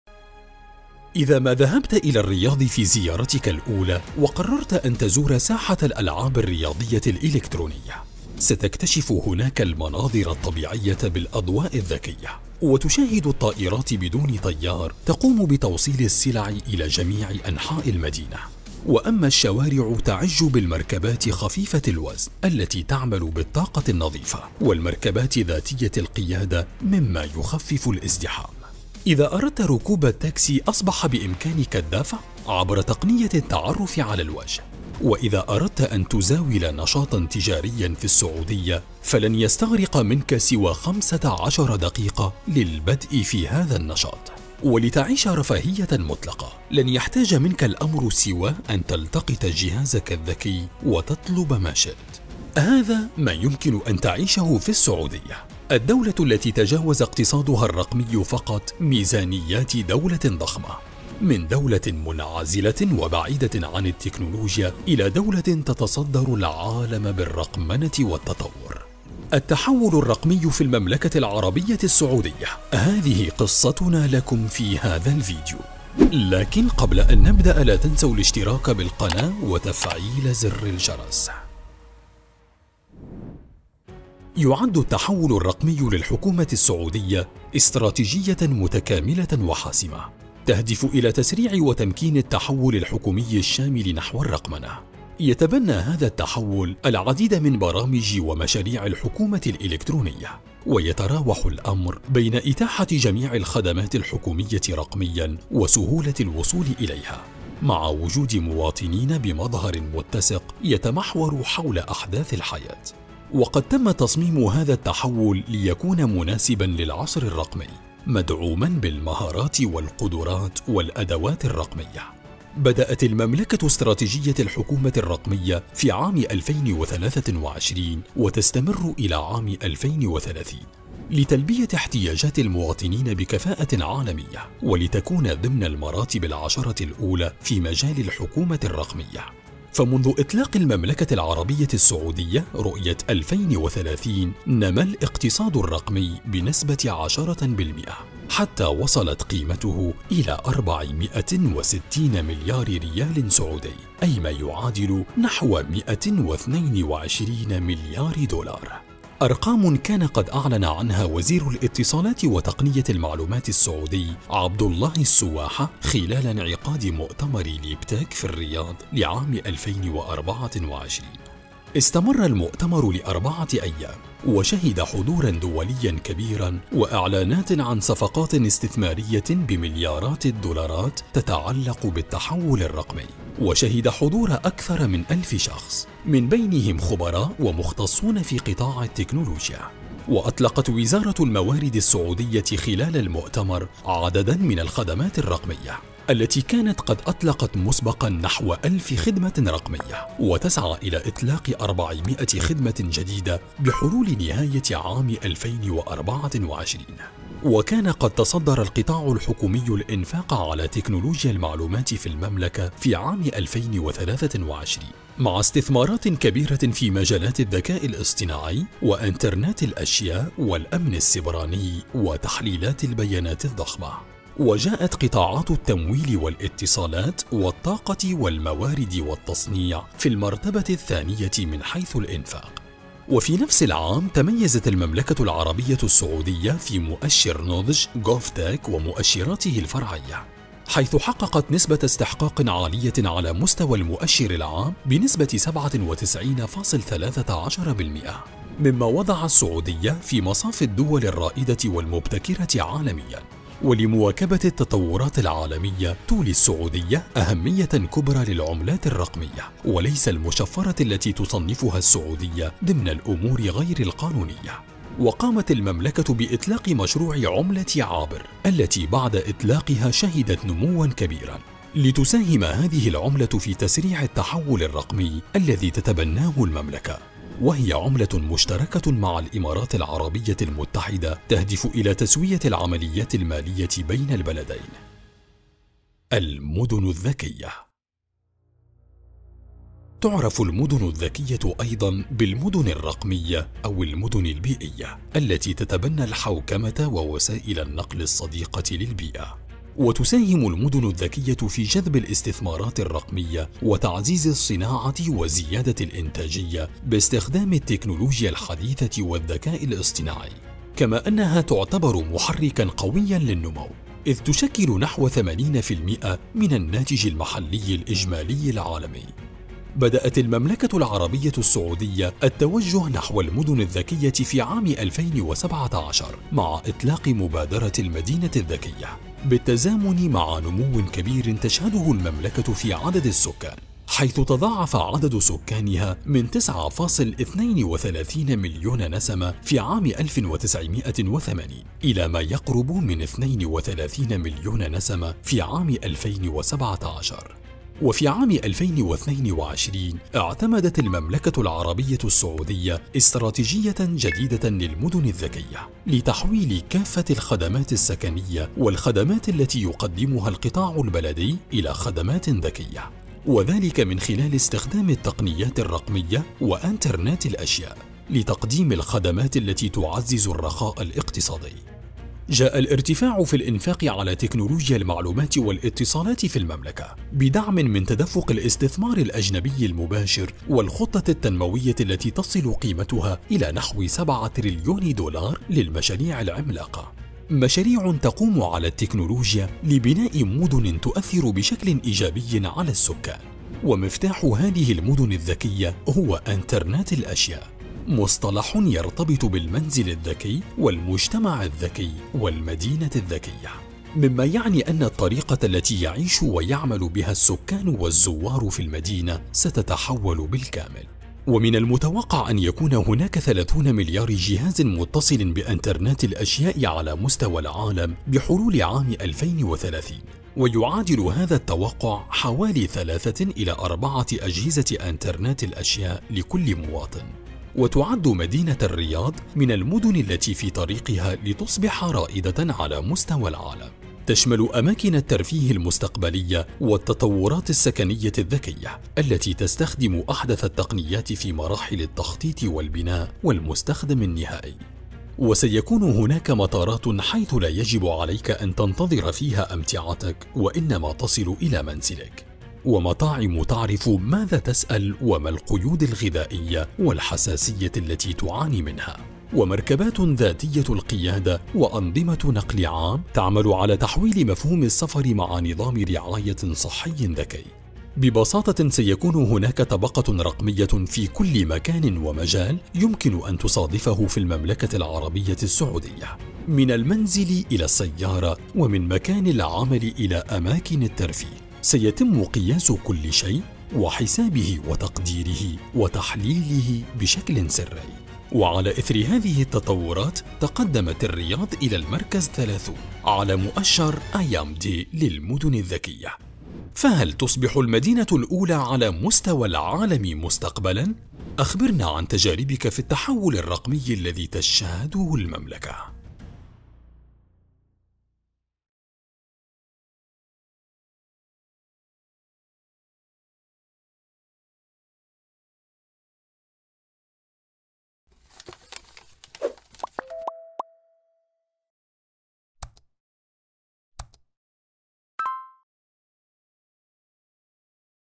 السعودية الرقمية البديلة.. اقتصاد تقني بالمليارات نحو عالم جديد
التحول الرقمي في المملكة العربية السعودية استراتيجية جديدة تبنتها المملكة في سبيل الوصول إلى أفضل نسخة رقمية حول العالم فإلى أين وصلت المملكة في هذا المجال وماهو حجم الاقتصاد الرقمي في السعودية
◀توضيح هام :
00:00 المقدمة
1:23 التحول الرقمي السعودي
2:27 الاقتصاد الرقمي
4:10 العملة الرقمية السعودية
4:47 المدن الذكية
6:15 المشاريع العملاقة
7:10 مدينة الرياض